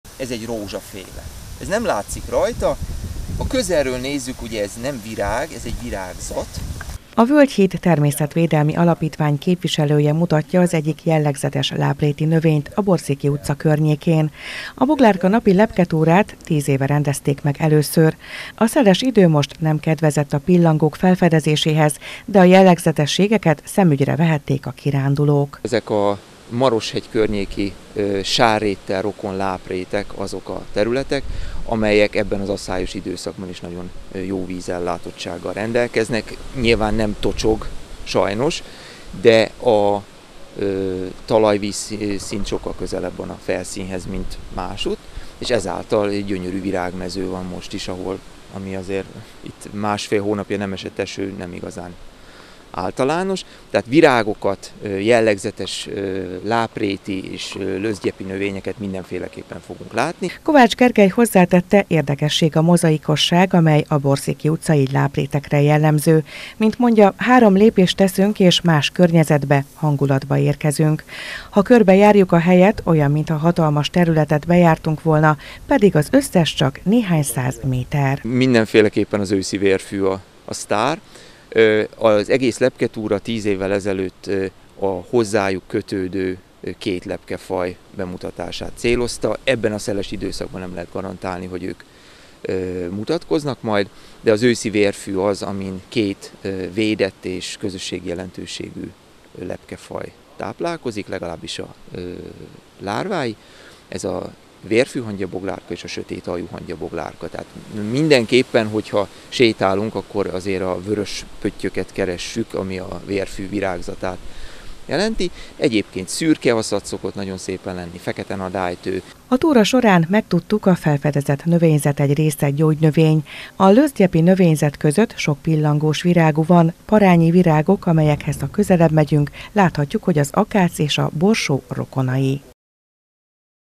BOGLÁRKA-NAPI LEPKETÚRA MAROSHEGYEN
Boglárka-napi lepketúrára várták a kirándulni vágyókat szombaton reggel Maroshegyen. A szeles idő miatt, ha pillangókat nem is nagyon láthatták, de a láprétek növény- és állatvilágát felfedezhették a résztvevők.